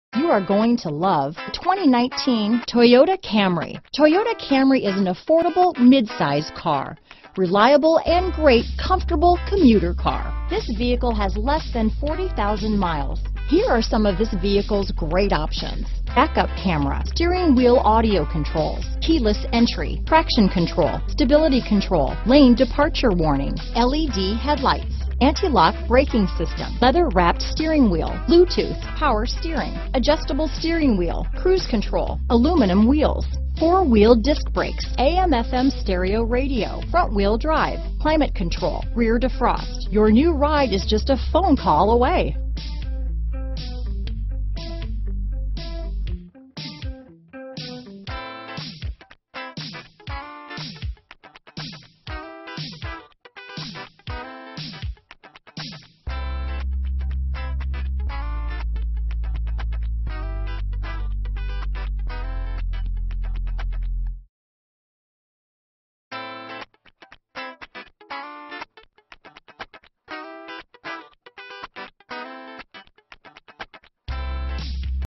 2019 Toyota Camry Hattiesburg, Lumberton, Sumrall, Ellisville, Wiggins, MS P6271
Predawn Gray Mica Used 2019 Toyota Camry available in Hattiesburg, Mississippi at Toyota of Hattiesburg. Servicing the Lumberton, Sumrall, Ellisville, Wiggins, MS area.